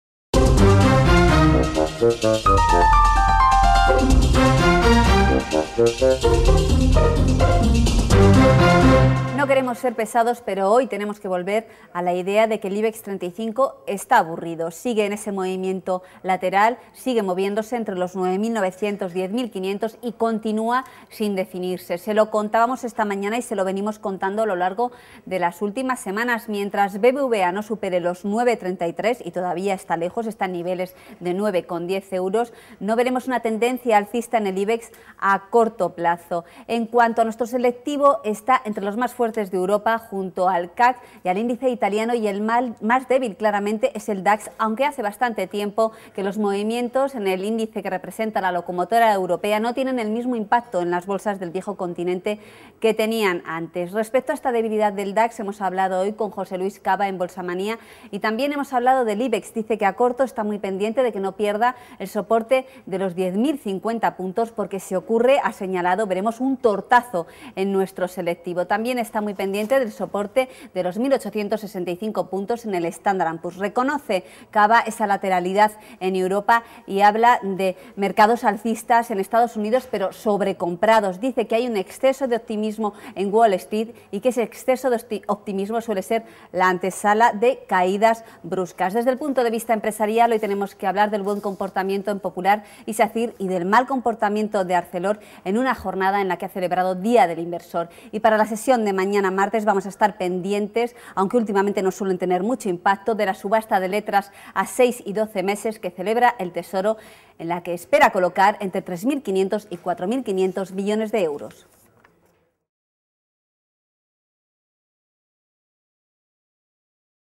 Radiografía de la sesión: El Ibex está... aburrido y pesado
El Ibex está aburrido. Está lateral y algo volátil dentro de la lateralidad pero, sobre todo, está aburrido. Aunque la semana pasada rompiera resistencias, no veremos un movimiento determinante al alza mientras BBVA no supere los 9,33. A la espera de que esto ocurra, el experto independiente José Luis Cava aconseja vigilar a corto plazo los 10.050.